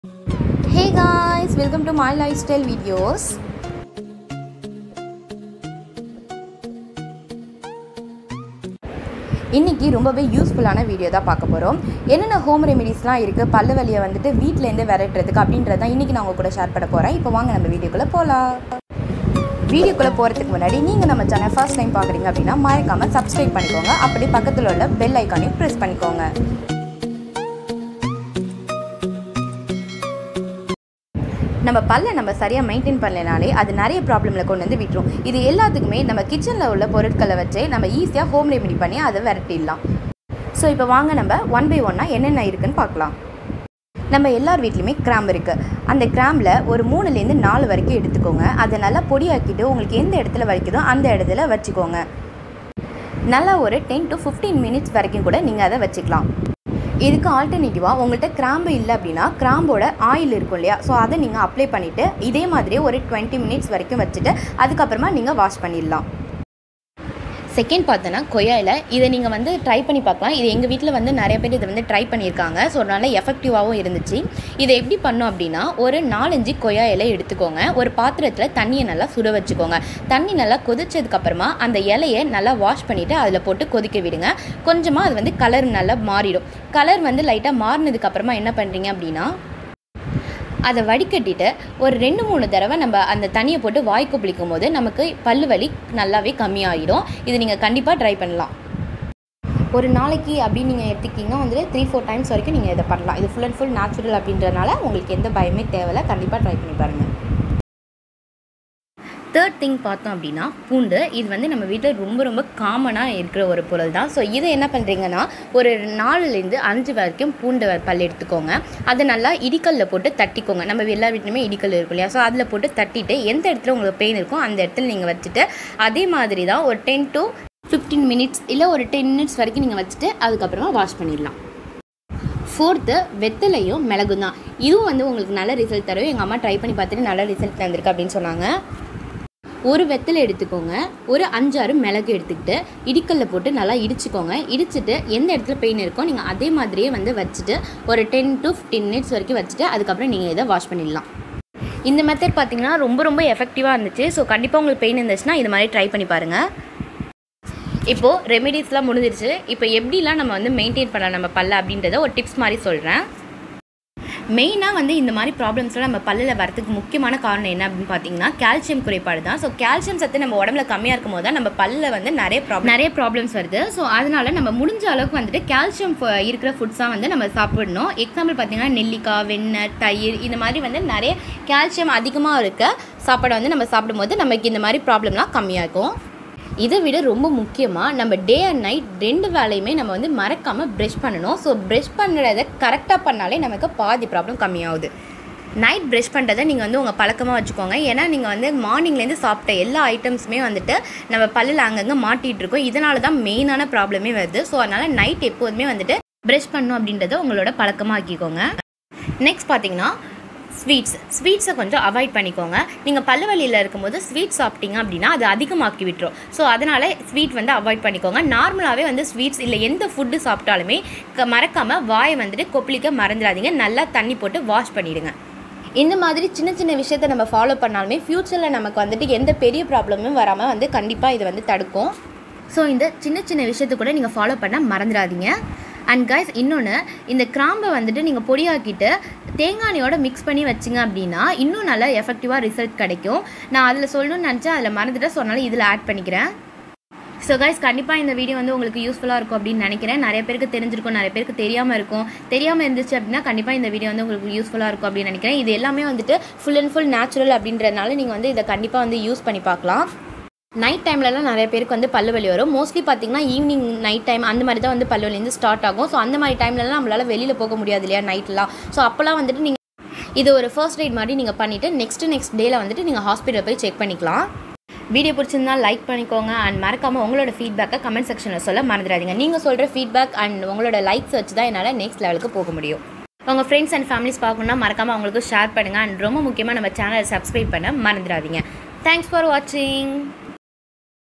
Home remedies for Toothache🦷||Tips for maintaining our teeth🤔🦷👍
Hy guys!!🙋‍♀️

Wlm 2 MY LIFESTYLE VIDEOS!!!🙋‍♀️🙏

➡️Here im giving some home remedies for toothache..😥🦷 hope tis video useful for u guys🤔✌tis is  💯 natural&very simple remedies✅
➡️Maintaining our teeth is 1step to avoid many problems🤟🦷
➡️If u have any doubt for making tis remedies pls ask me in the command section i will do clear videos🎥 for ur doubts✔

Command📝
Keep watching👀&supporting🙏💪
Thq 4 watching🙏